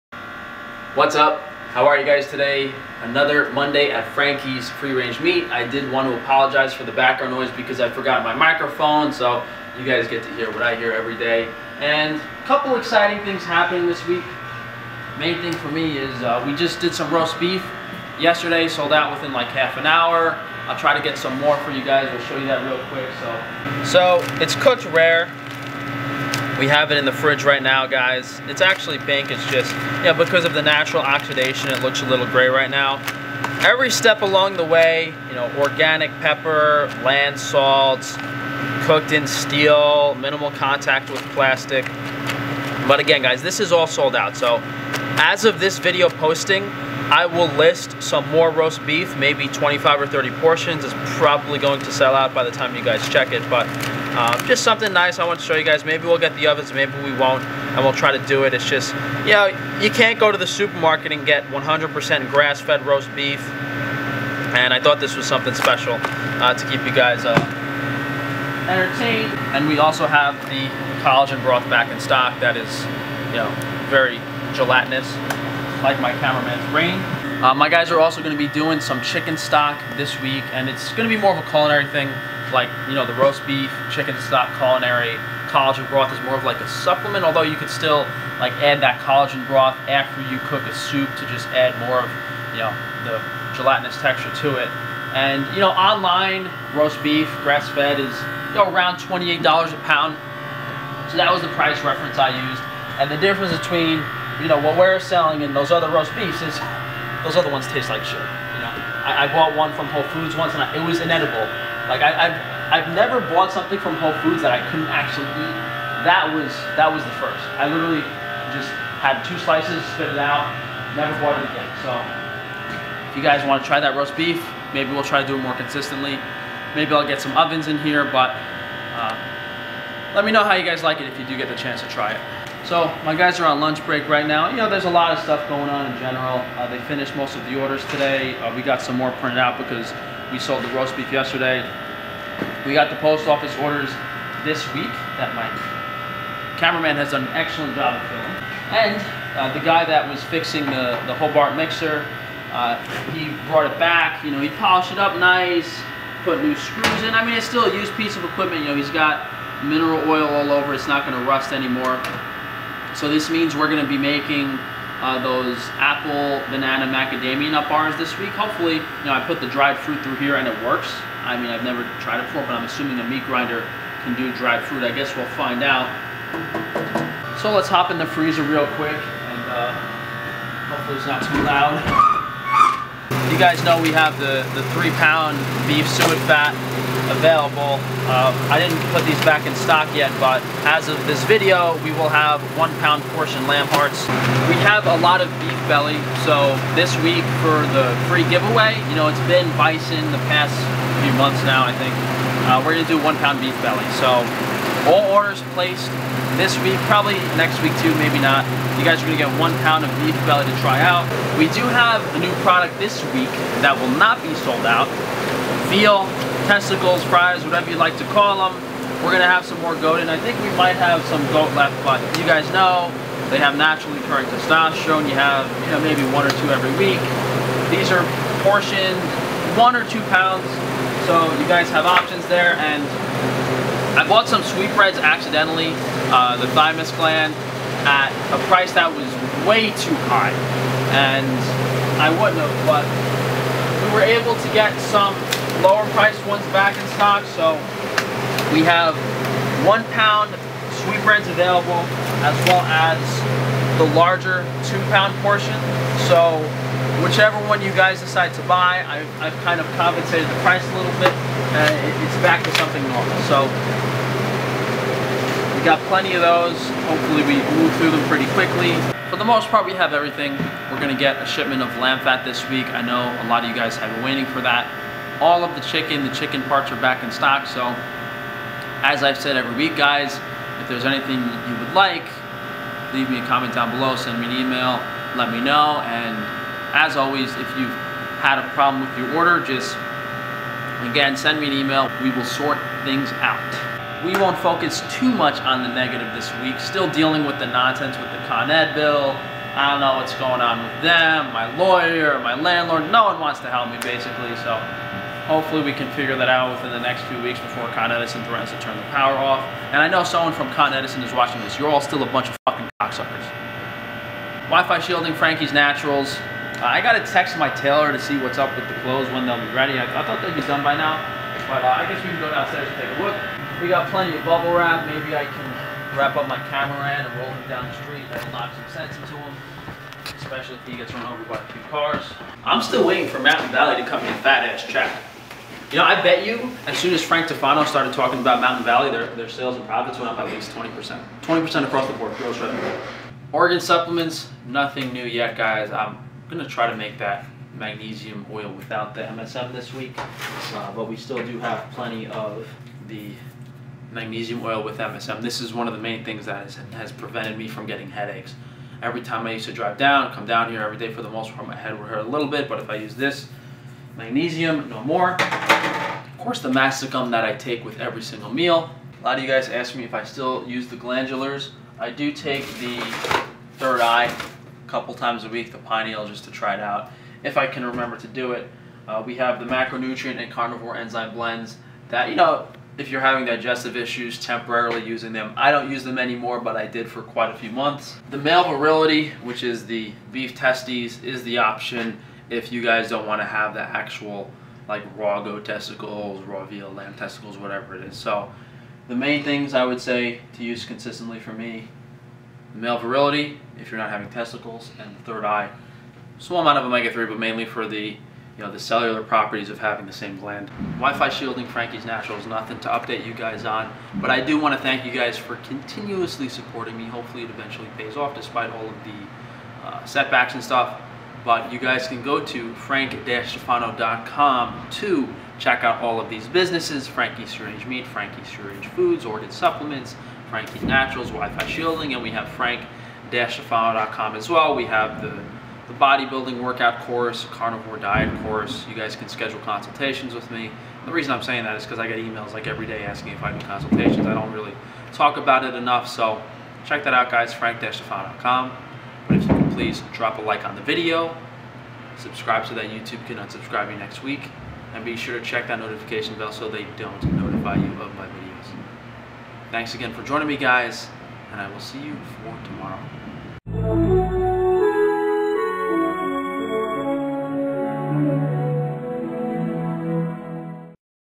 ROAST BEEF! CHICKEN STOCK! Frankie's Free Range Meat Vlog
00:00 - Some lovely background noise
00:23 - Roast beef!
02:12 - Meat grinder is BRAND SPANKIN NEW
02:57 - Into the depths
04:53 - Con Edison blow me
05:14 - What else is going on?
05:43 - Organ Supplements
06:50 - Wrapping things up